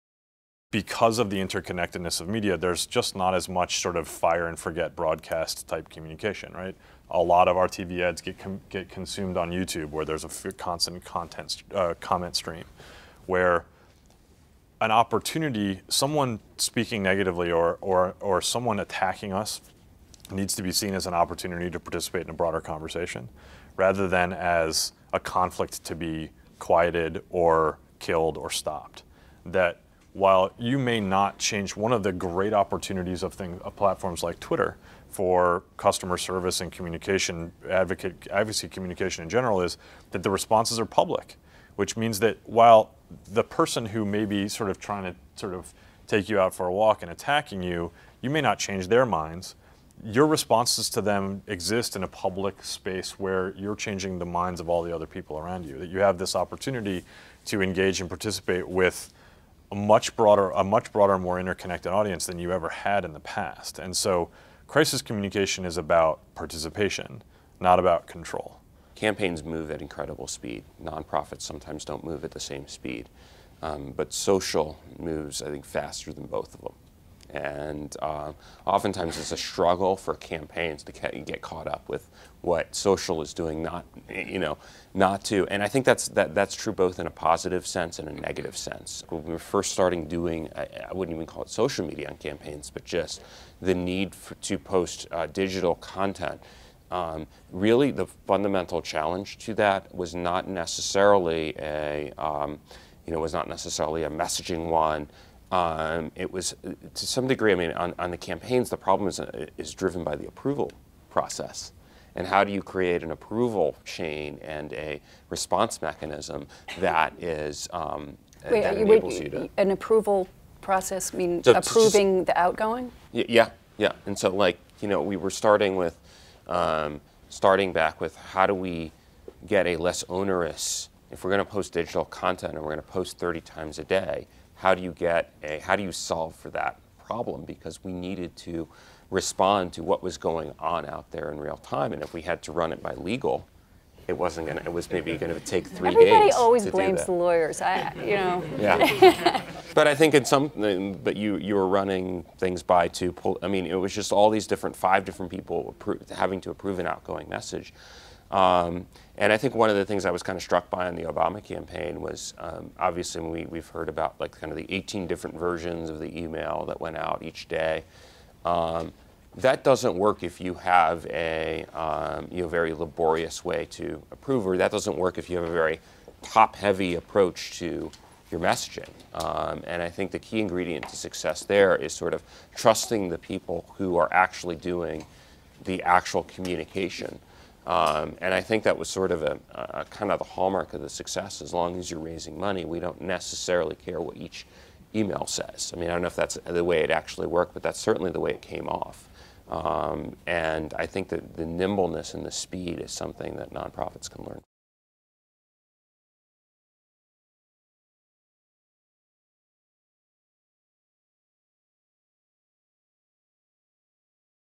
How do you handle negative conversations?
Patrick Ruffini and Michael Slaby discuss how nonprofits can utilize new digital tools.